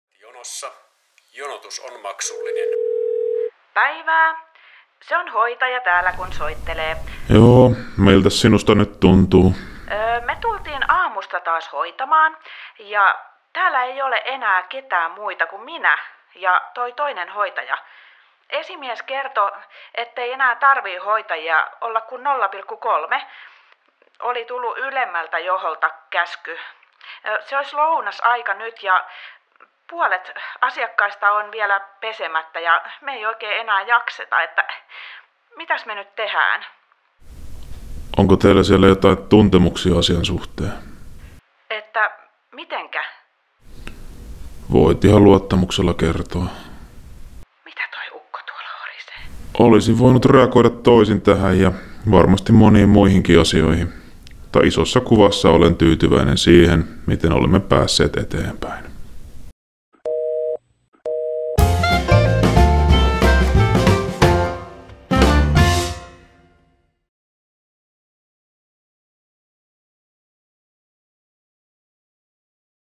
Osa 2. Hoitaja pulassa – henkilöstömitoitukset pielessä | SuPer-liitto
Hoitaja on jälleen pulassa.  Tällä kertaa hän soittaa auttavaan puhelimeen, kun työnantaja uskottelee mitoitusten jälleen kutistuneen.

Osaako maksullinen ”tukiluuri” auttaa? Saatko rahoillesi vastinetta, vai valuvatko maksusi konsultin pohjattomaan taskuun?
SuPerin jäsenenä saat asiantuntevaa neuvontaa ja lakimiehen palvelut ilman omavastuuta.